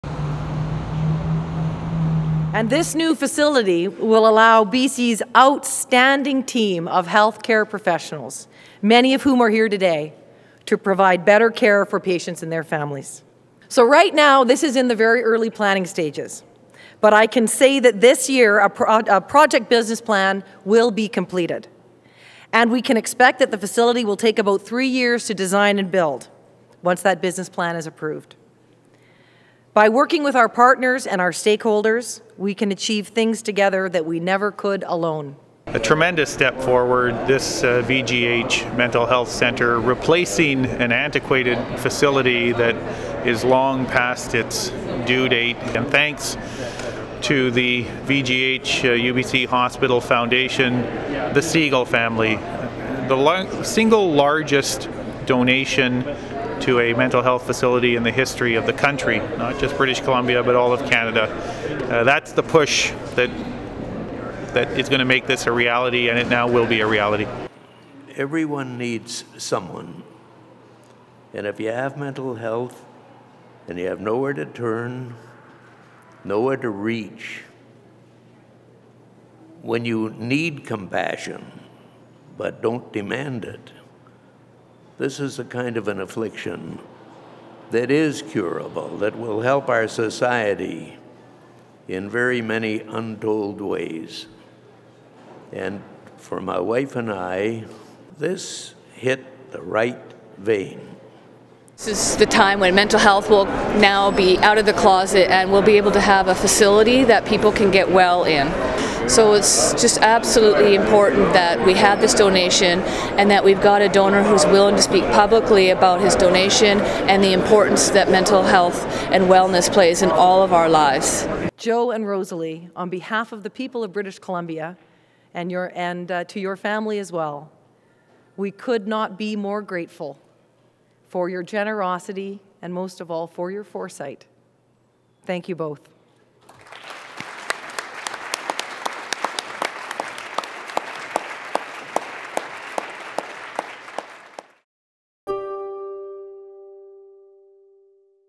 Province Proceeds with Vancouver General Hospital Mental Health Centre
Premier Christy Clark today announced a new mental health facility at Vancouver General Hospital (VGH), as a part of Budget 2012. The new facility will focus on a patient-centred therapeutic environment that meets the highest standard of modern psychiatric care for patients. In addition to funding from the province and the VGH & UBC Hospitals Foundation, Vancouver philanthropists Joseph and Rosalie Segal have given $12 million towards the project, one of the largest-ever personal gifts for mental health in Canada.